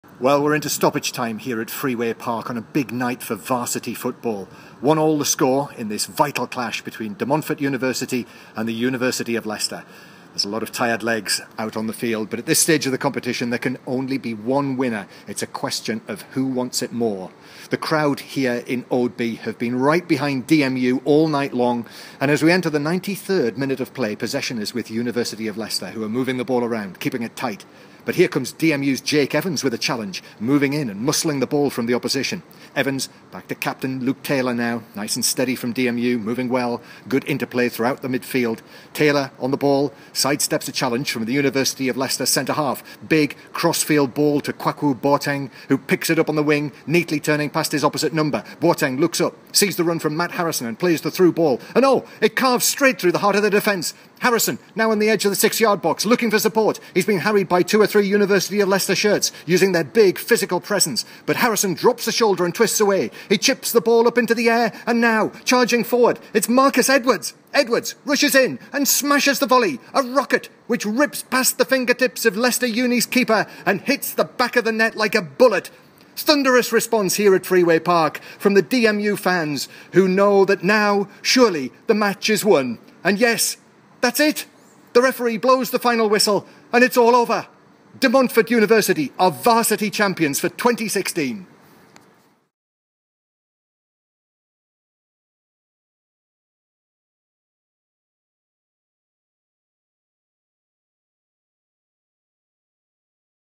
John Murray Varsity 2016
What would it sound like if DMU won Varsity? Who better than BBC football commentator John Murray to talk us through it?! Have a listen ... it's brill :)